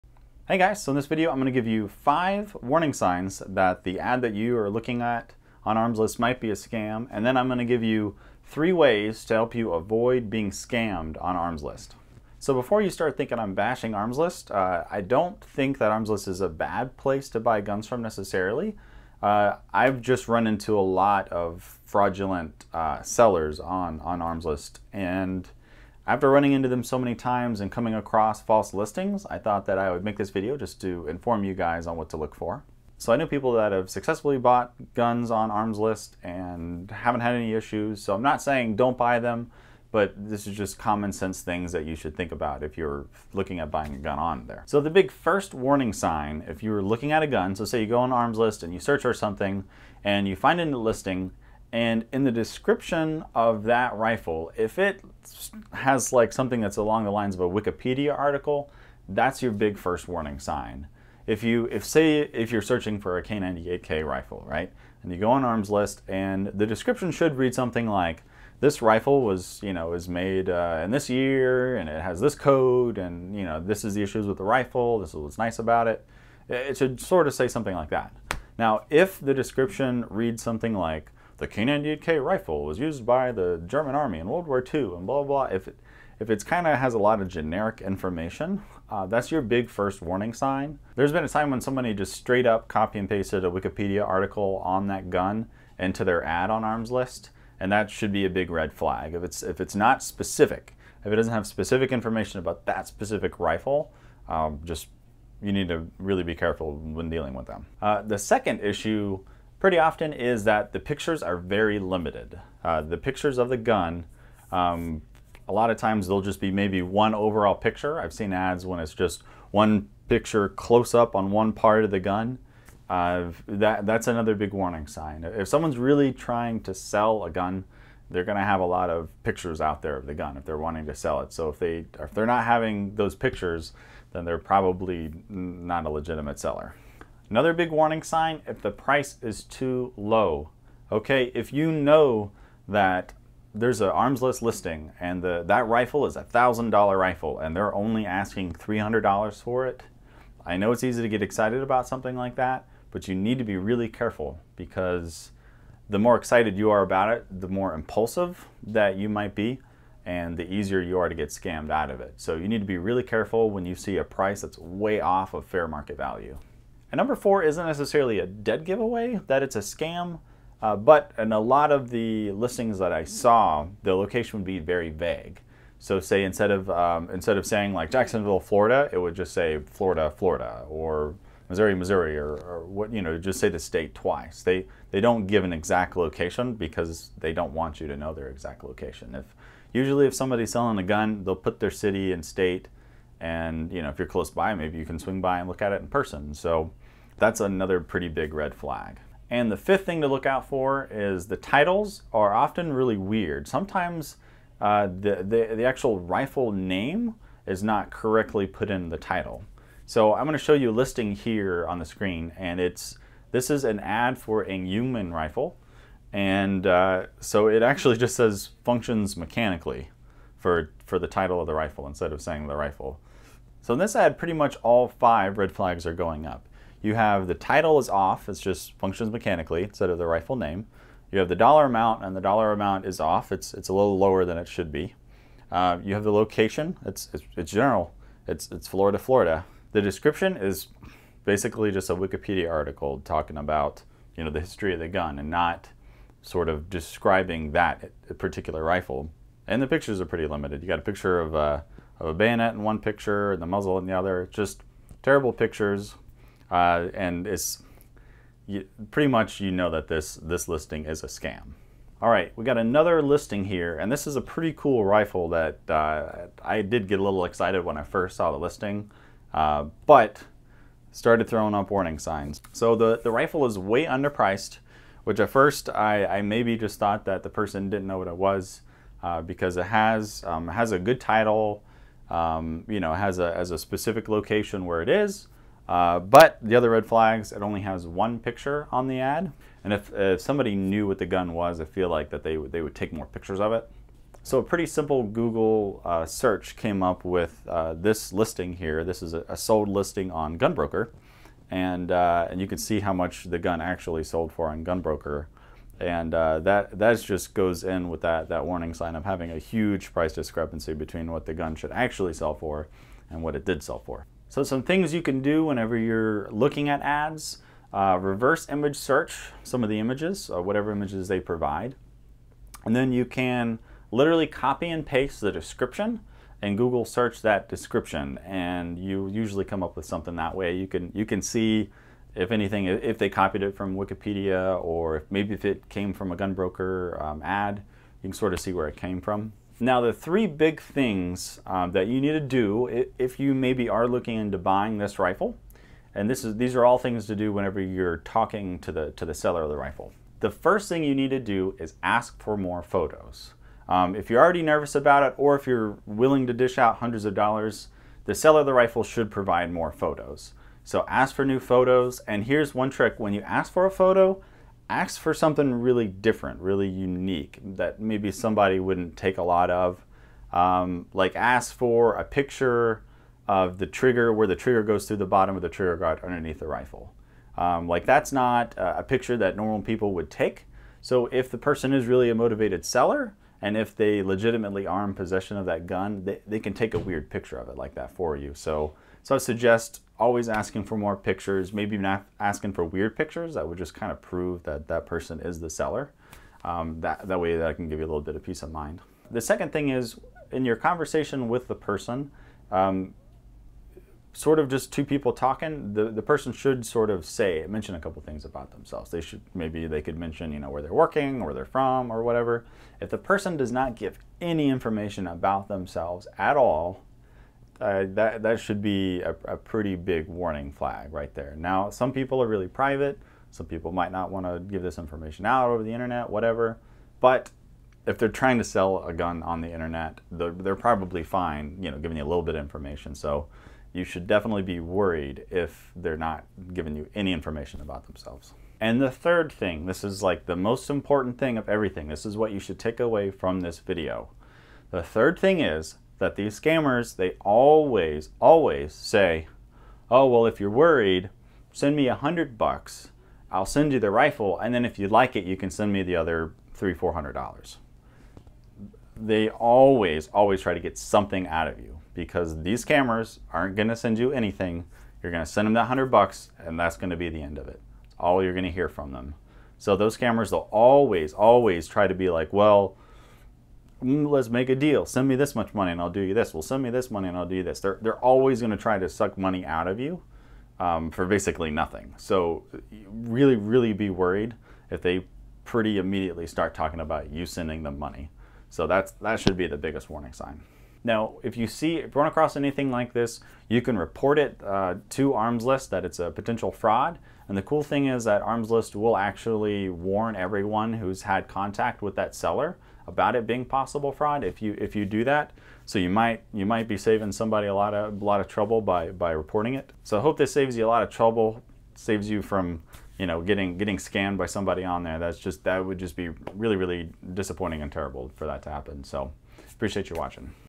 8 Tips to Keep You from Getting Scammed on Armslist
This video is an Armslist scam alert. I have recently stumbled upon a lot of listings on Armslist that turned out to be frauds. I hope the tips in the video can help people not get conned. There are of course other types of scams including the sending of a much larger amount and asking for money back.